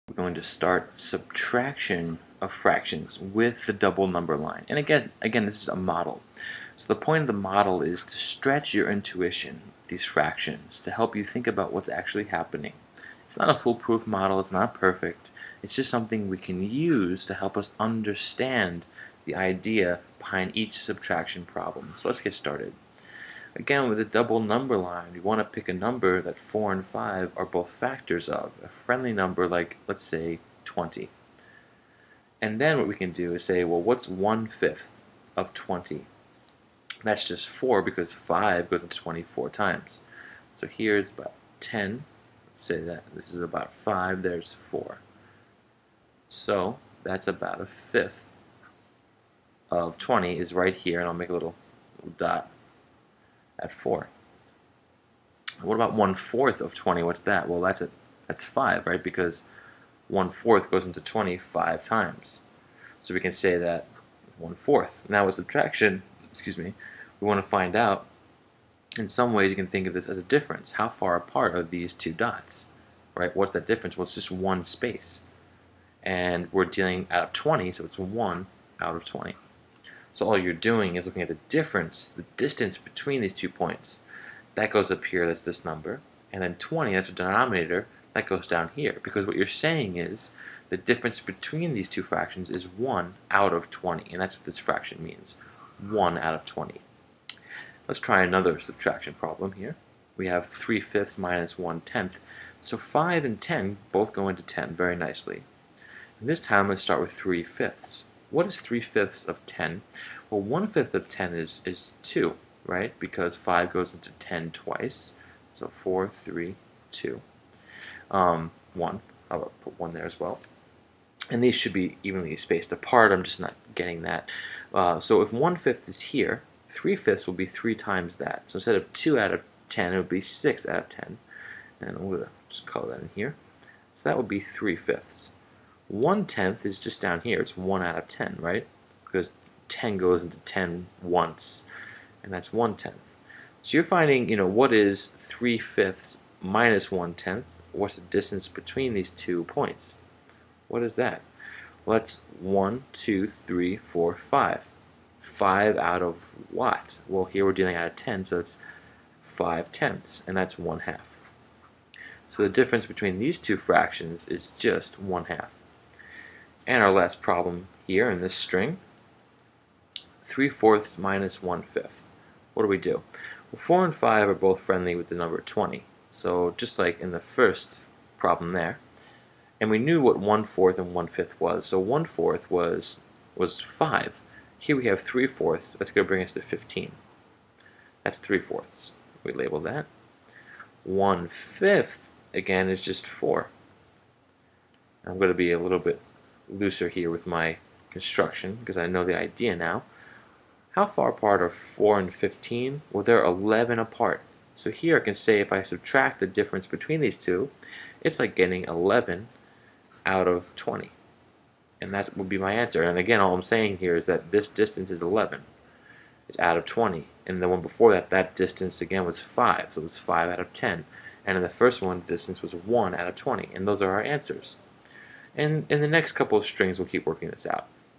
Using a Double Number Line String 6
Using a double number line to understand fraction sums